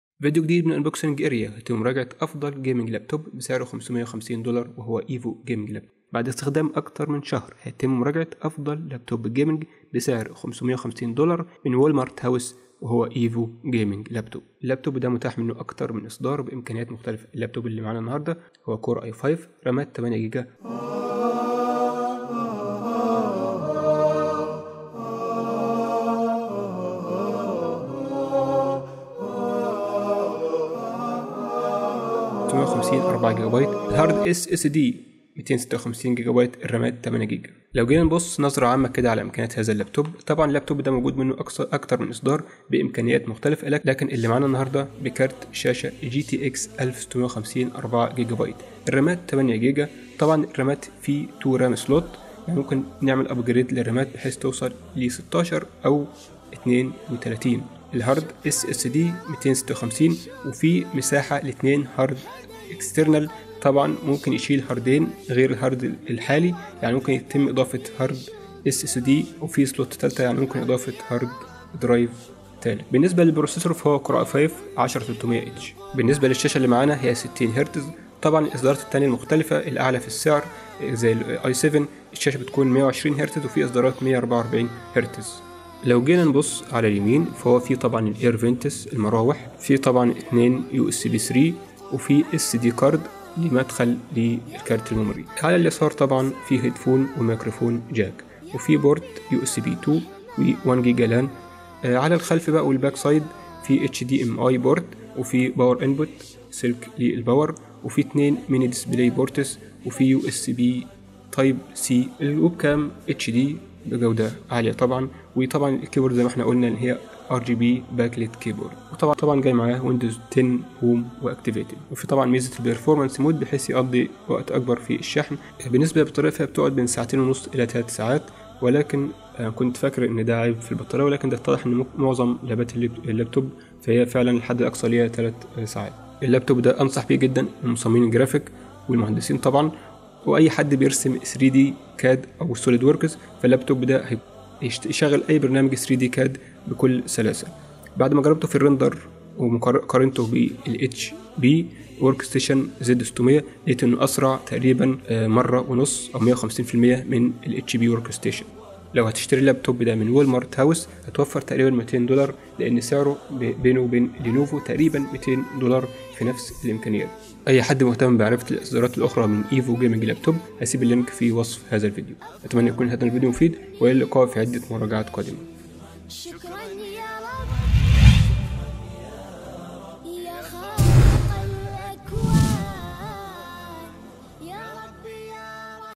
أرخص وأفضل لابتوب للمهندسين | لابتوب Gaming بسعر 550$
Evoo Gaming 15.6” Laptop, FHD, 60Hz, Intel Core i5-10300H Processor, NVIDIA GeForce GTX 1650, THX Spatial Audio, 256GB SSD, 8GB RAM, RGB Backlit Keyboard, HD Camera, Windows 10 Home, Black, EG-LP10-BK

يمكنك شراء المنتج من هذا اللينك اذا كنت خارج مصر: